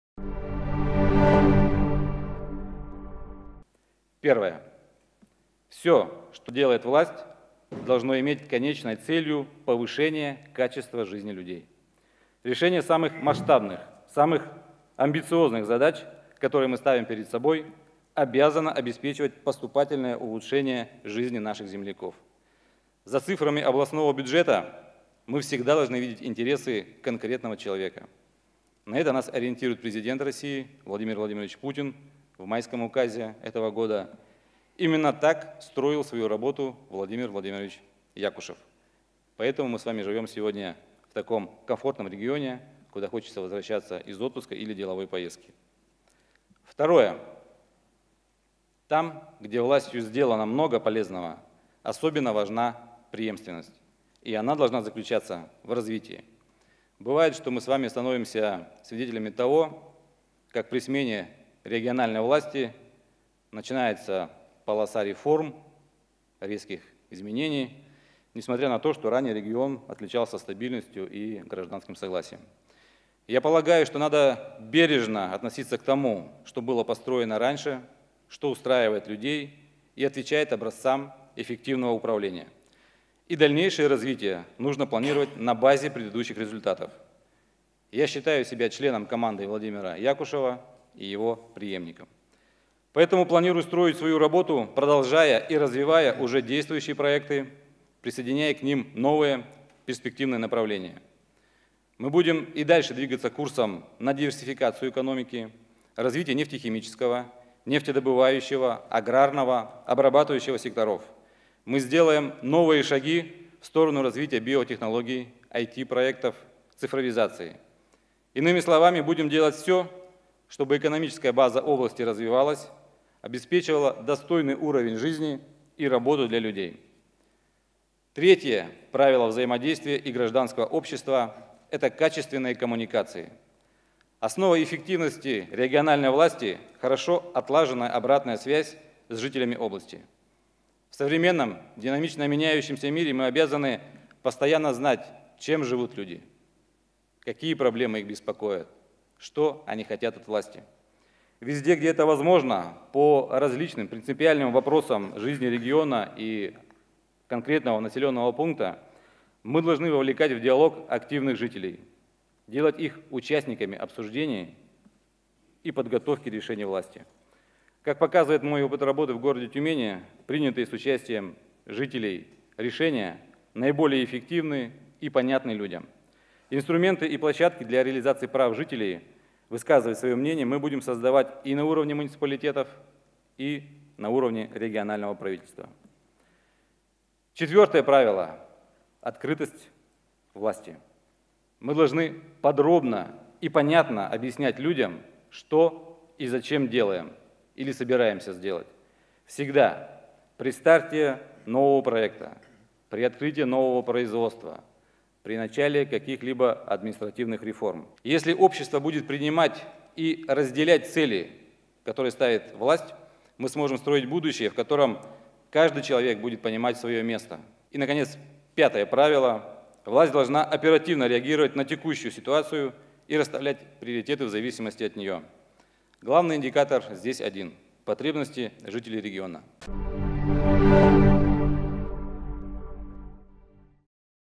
Александр Моор обозначил приоритеты на посту губернатора Тюменской области
Обязательства, которые на выборах губернатора Тюменской области в 2014 году взяли на себя Владимир Якушев и региональное отделение партии "Единая Россия" – выполнены. Об этом заявил врио главы региона, кандидат от регионального отделения партии "Единая Россия" на пост губернатора Александр Моор на партийной конференции в тюменском технопарке 18 июля.

"Тюменская область продолжает лидировать в Российской Федерации по ключевым показателям социально-экономического развития. За последние четыре года решены многие сложные для людей вопросы. Существенно увеличена заработная плата работникам бюджетной сферы. Тюменская область в числе первых в стране выполнила программу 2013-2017 годов по переселению людей из аварийного фонда в пределах установленного контрольного срока. В настоящий момент составлена и реализуется новая программа", - отметил Александр Моор, выступая перед делегатами конференции.

Еще один важный результат, который назвал врио губернатора - это внедрение механизма помощи дольщикам недостроенных жилых домов, в результате чего в свои квартиры вселились более 7 тыс. семей.

"Мы уверенно приближаемся к решению проблемы дефицита школ. Об этом могут сказать очень немногие регионы. В ближайшие годы мы полностью перейдем на обучение детей в одну смену, освободив им время для дополнительного развития", - подчеркнул он.

За выполнением важнейших социальных задач, безусловно, стоит серьезная экономическая база, добавил врио губернатора. За последние 10 лет в Тюменской области появились 50 новых заводов, среди которых и крупные предприятия межрегионального значения, и небольшие производства.

Александр Моор уверен, что главная задача нового губернатора - не останавливаться в развитии, двигаться вперед. Он сформулировал несколько правил, на основе которых считает правильным строить отношения власти с гражданским обществом.

"Первое. Все, что делает власть, должно иметь конечной целью повышение качества жизни людей. Второе. Там, где властью сделано много полезного, особенно важна преемственность. И она должна заключаться в развитии. Я считаю себя членом команды Владимира Якушева и его преемником. Третье правило взаимодействия власти и гражданского общества – качественные коммуникации. Основа эффективности региональной власти – хорошо отлаженная обратная связь с жителями области. Четвертое правило – открытость власти. Мы должны подробно и понятно объяснять людям, что и зачем делаем или собираемся сделать. Люди должны понимать, какой эффект предпринимаемые действия дадут области и ее жителям – сегодня, завтра, через несколько лет. Пятое правило - власть должна оперативно реагировать на текущую ситуацию и расставлять приоритеты в зависимости от нее. Главный индикатор здесь один – потребности жителей территории", - завяил Александр Моор.